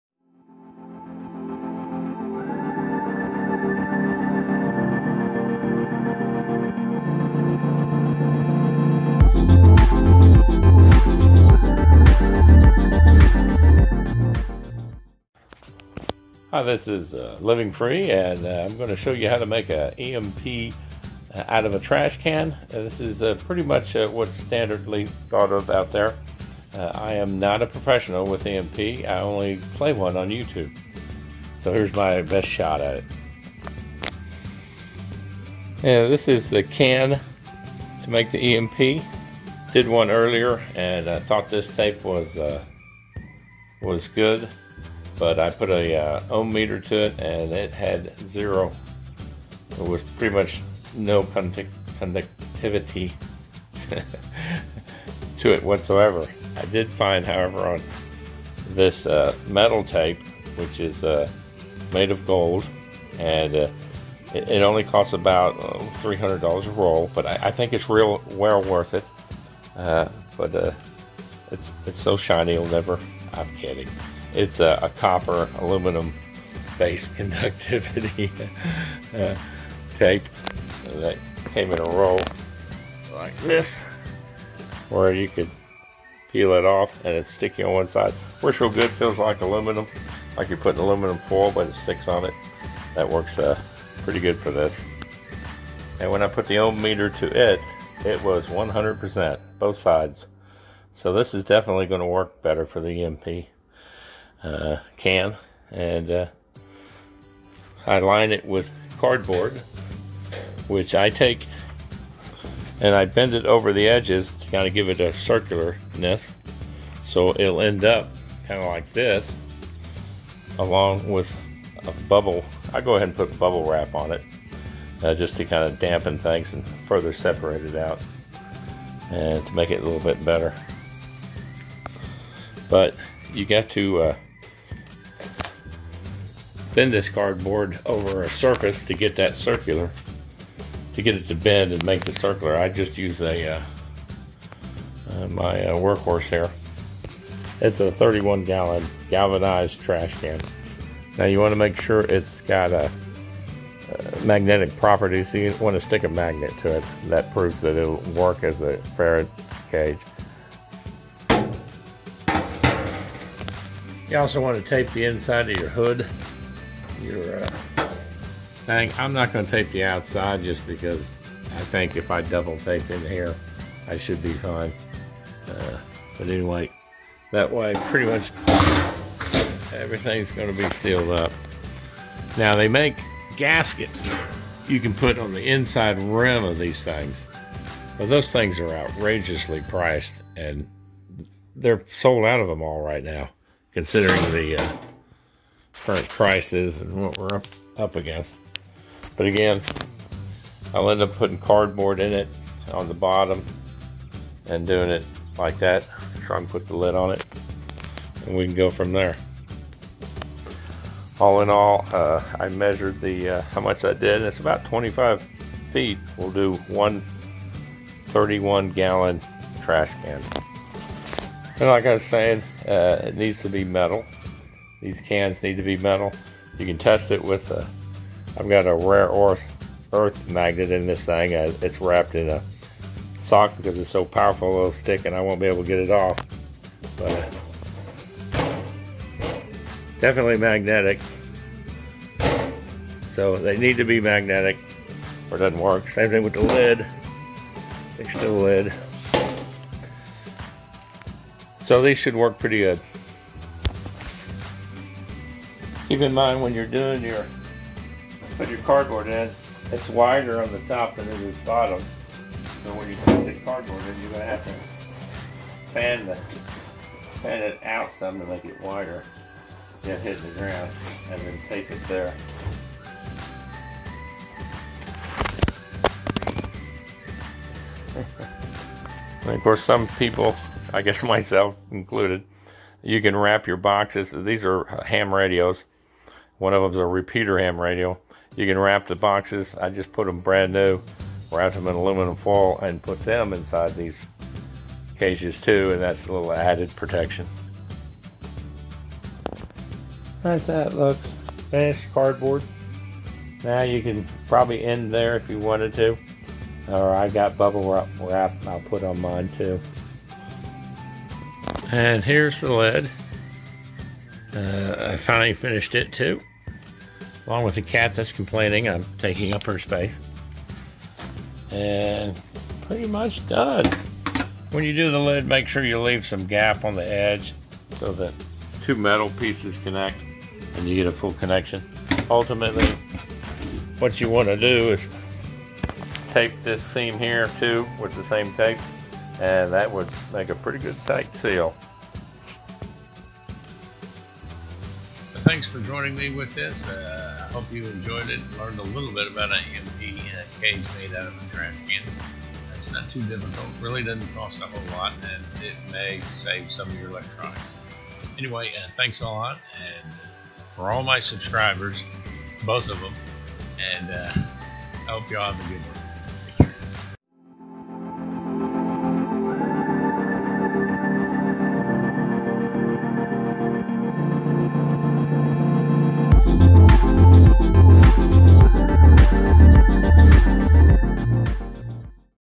Making an EMP Cage from a Trash Can
A brief video about how to make a EMP Cage to protect some of your electronics from an ElectroMagnetic Pulse waves.